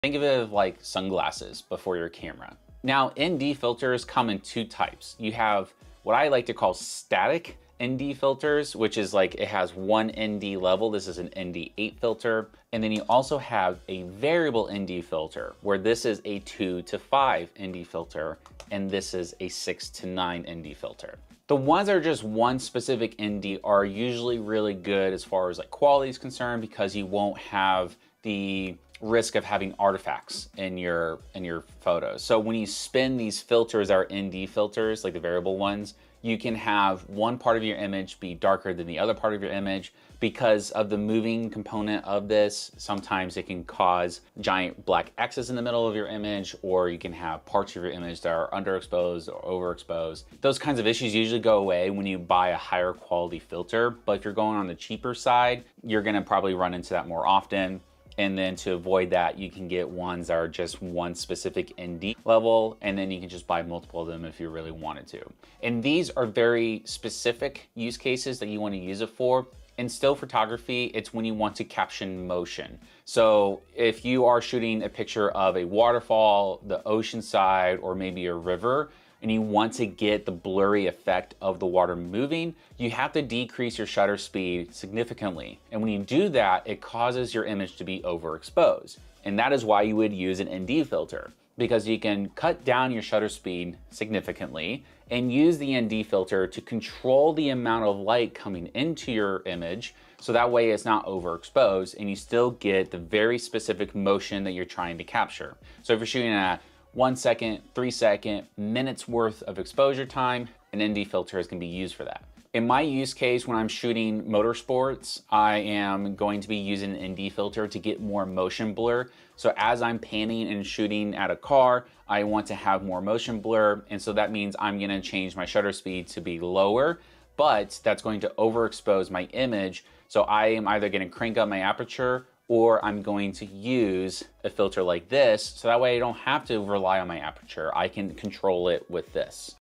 ND Filters: Your Camera's Sunglasses Explained!
We discuss ND filters, both static and variable, and their uses in photography. We explore capturing motion blur in waterfalls and motorsport. Learn how ND filters control light for perfect exposures.